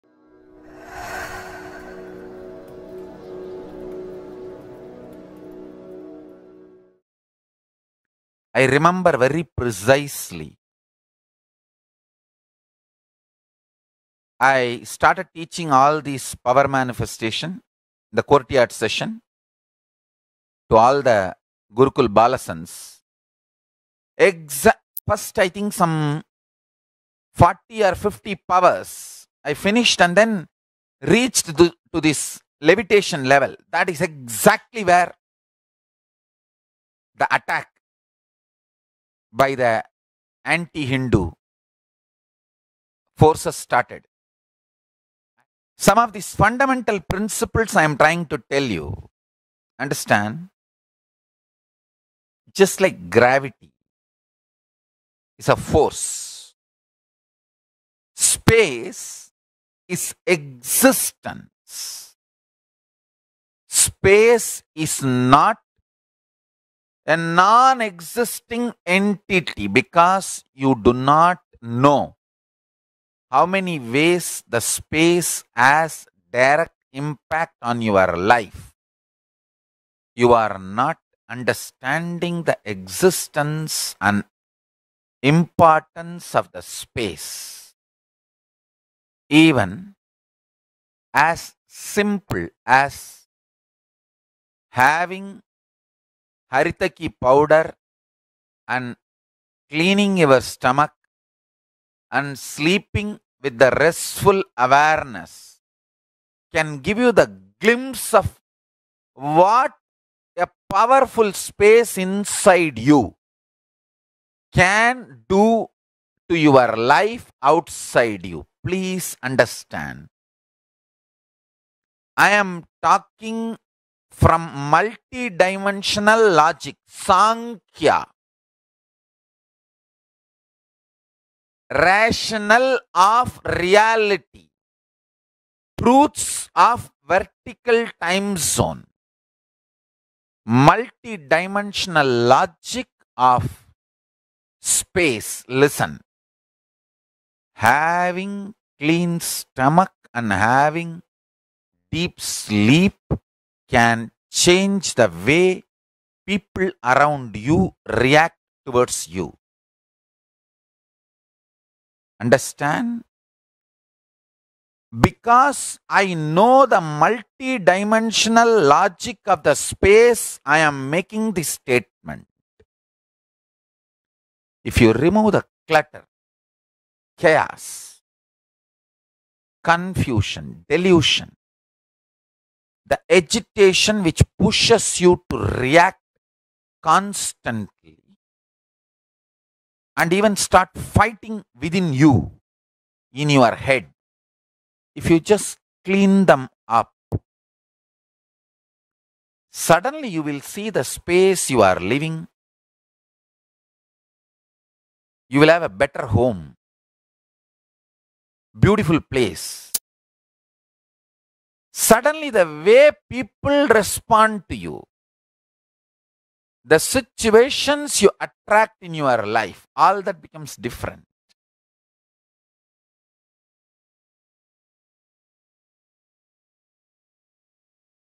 The Importance of Having a Powerful Space
I remember very precisely I started teaching all this power manifestation, the Courtyard session to all the Guruku Balasanths, exa.., first I think some forty or fifty powers, I finished and then reached to this levitation level that is exactly where the attack by the anti-Hindu forces started. Some of these fundamental principles I am trying to tell you, understand, just like gravity is a force, space is existence, space is not a non-existing entity because you do not know how many ways the space has direct impact on your life. You are not understanding the existence and importance of the space even as simple as having haritaki powder and  cleaning your stomach and sleeping with the restful awareness can give you the glimpse of what a powerful space inside you can do to your life outside you. Please understand, I am talking from multidimensional logic, Sankhya, rational of reality, truths of vertical time zone, multidimensional logic of space, listen, having clean stomach and having deep sleep can change the way people around you react towards you. Understand, because I know the multidimensional logic of the space I am making this statement ‘ If you remove the clutter, chaos, confusion, delusion the agitation which pushes you to react constantly and even start fighting within you in your head, if you just clean them up, suddenly you will see the space you are living, you will have a better home, beautiful place, suddenly the way people respond to you, the situations you attract in your life, all that becomes different.